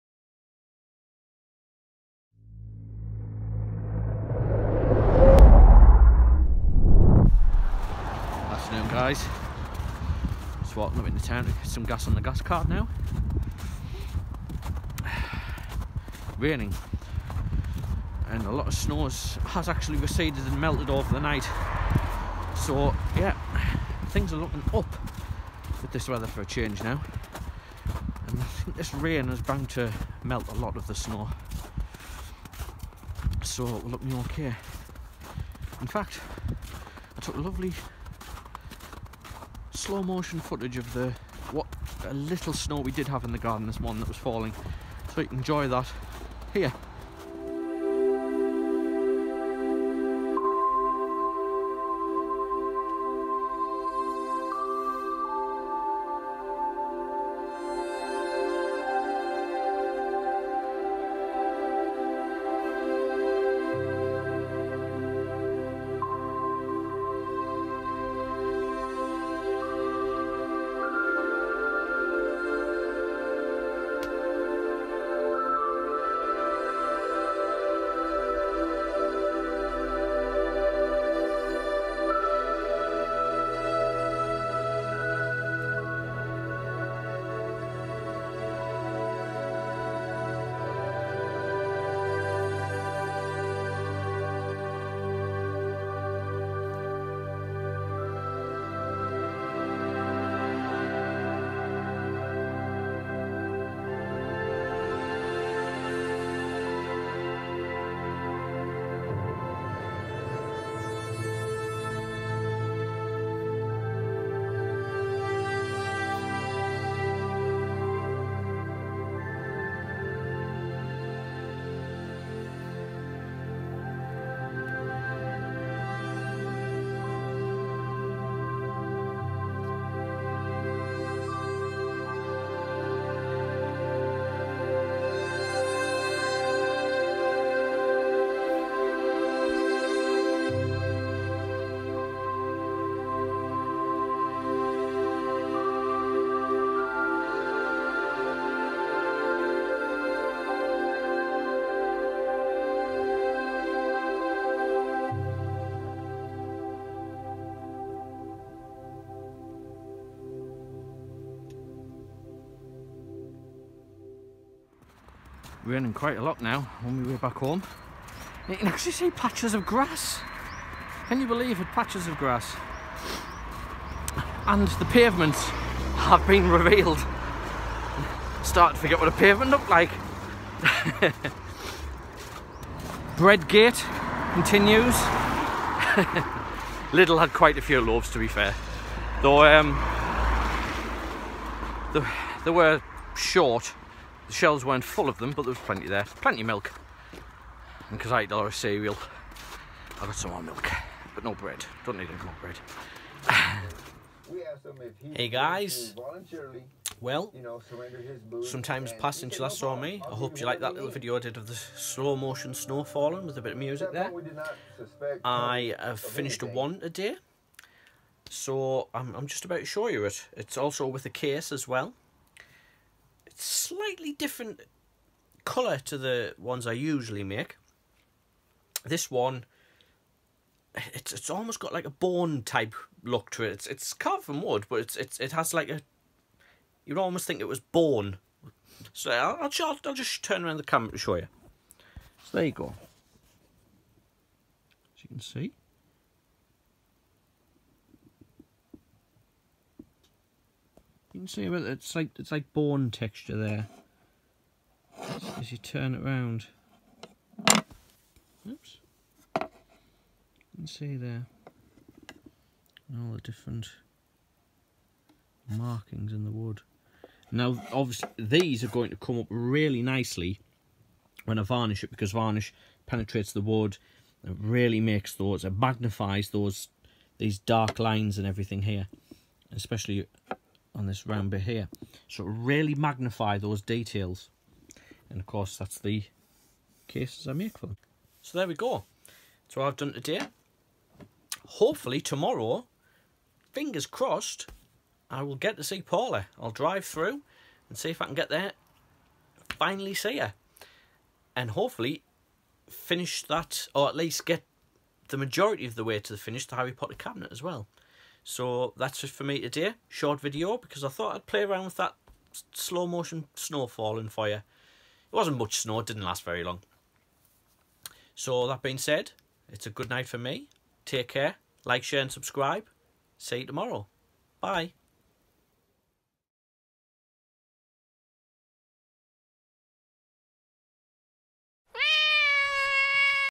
The snow begins to melt and I play around with my slo mo camera
So today was pretty quiet really. We did get some snow but it didn’t fall very long, however it looked so pretty as it did I decided to try my slo mo camera. I also managed to finish carving another wand.